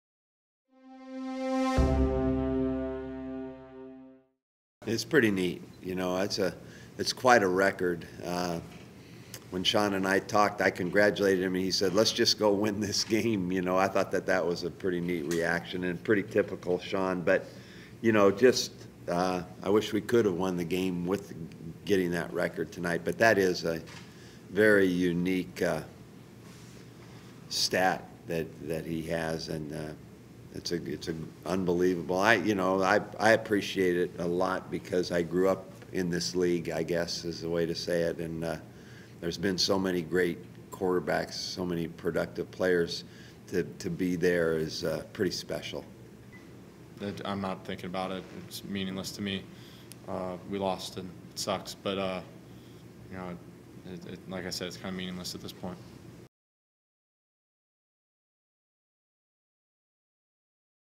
Oregon State Beavers QB Sean Mannion becomes Pac-12's all-time leading passer
Oregon State quarterback Sean Mannion and his head coach, Mike Riley, have differing reactions to his record breaking night in a 45-31 loss to Cal.